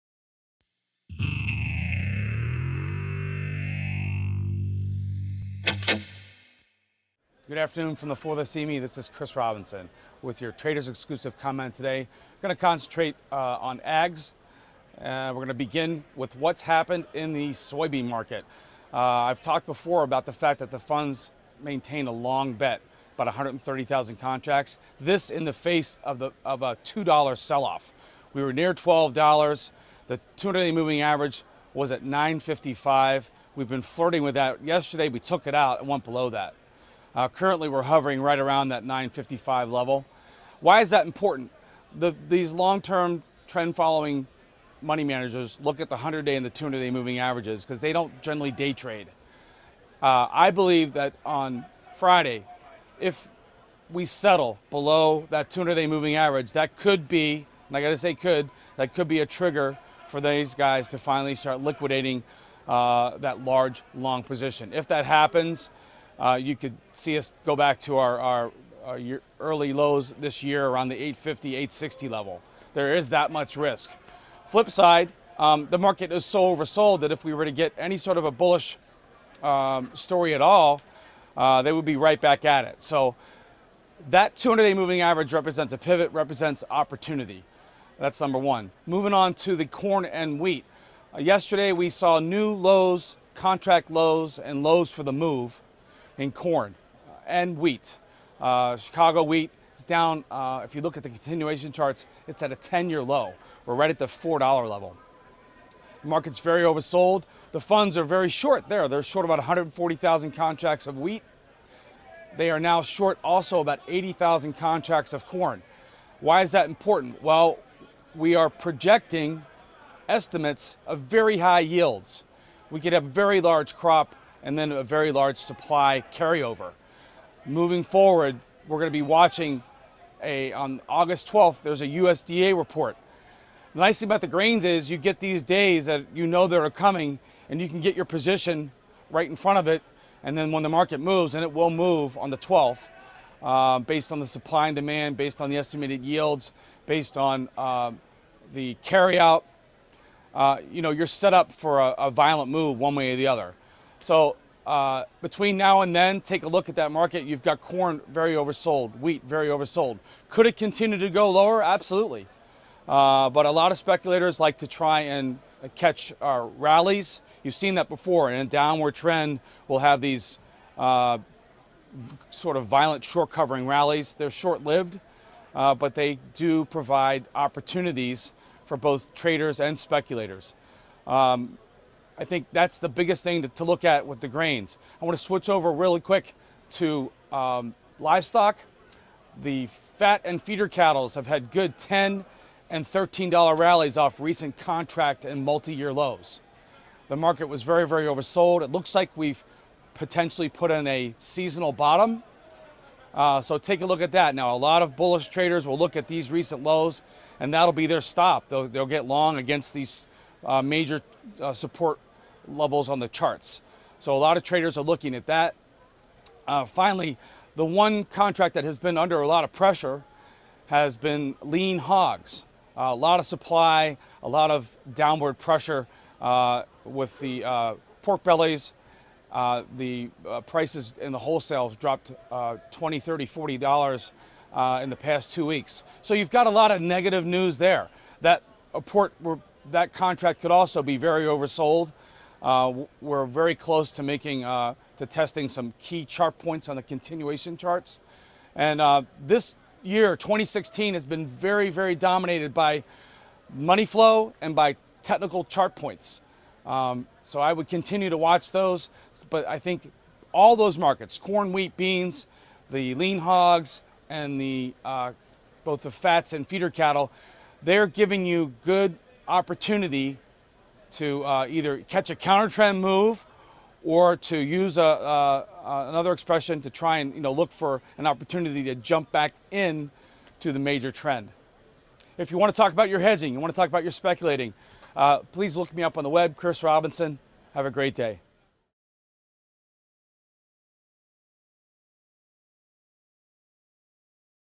Market Anticipates a Move After USDA Report
Soybean funds maintain a long bet in the face of a sell off. Market is oversold on wheat and short on corn.